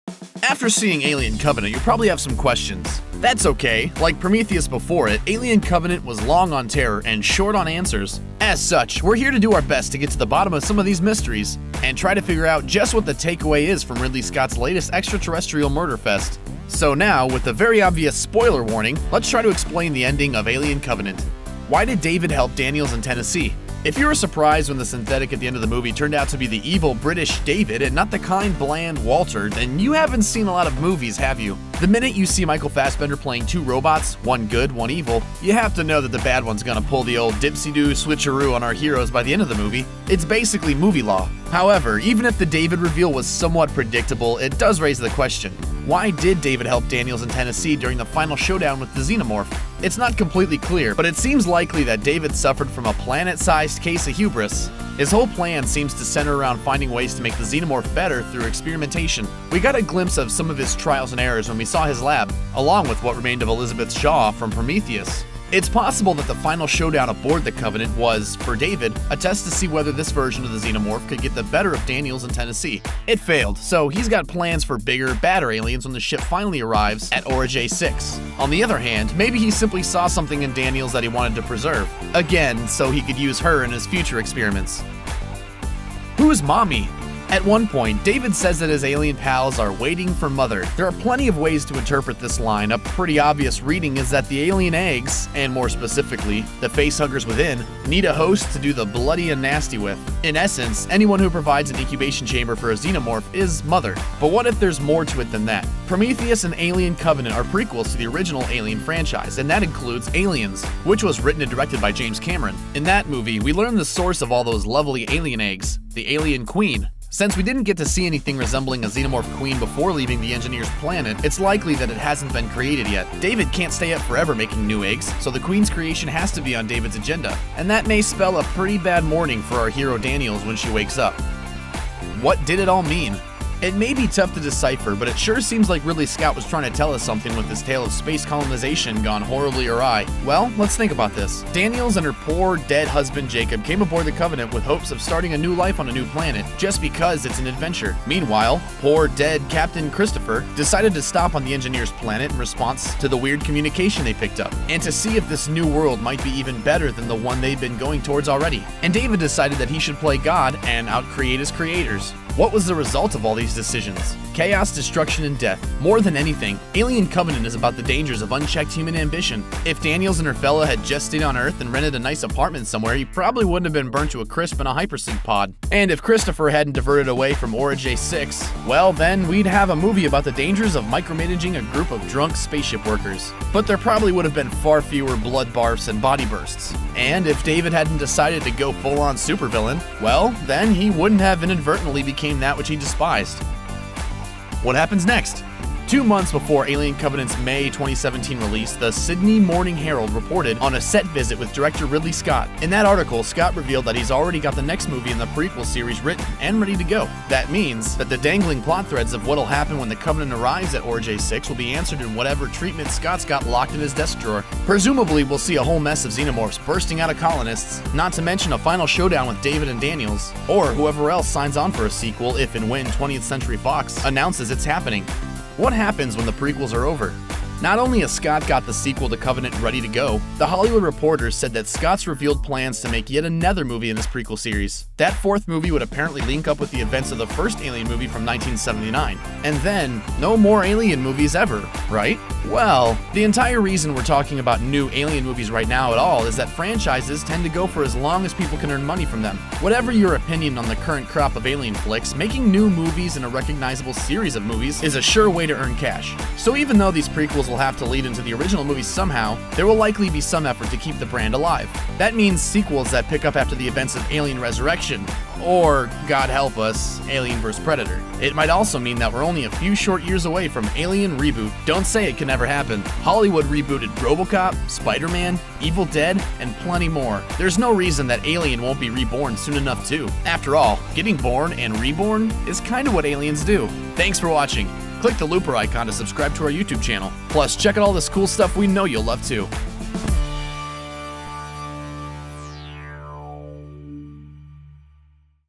The Ending Of Alien: Covenant Explained
Alien: Covenant is the most recent installment in the Alien series, and while it was pretty well-received by fans of the franchise, there are still some questions people had about the ending.

Some of the character decisions didn’t quite make sense to some audience members, and others wondered how everything ties into the mythology. Ridley Scott has said it won’t be the last film in the series, so there’s more that can be explored in another sequel, but if you want to know exactly what this one was trying to say, let’s take a look at the explanation of Alien: Covenant’s ending.

Why did David help Daniels and Tennessee? | 0:00
Who's mommy? | 1:25
What did it all mean? | 2:12
What happens next? | 3:14
What happens when the prequels are over? | 3:46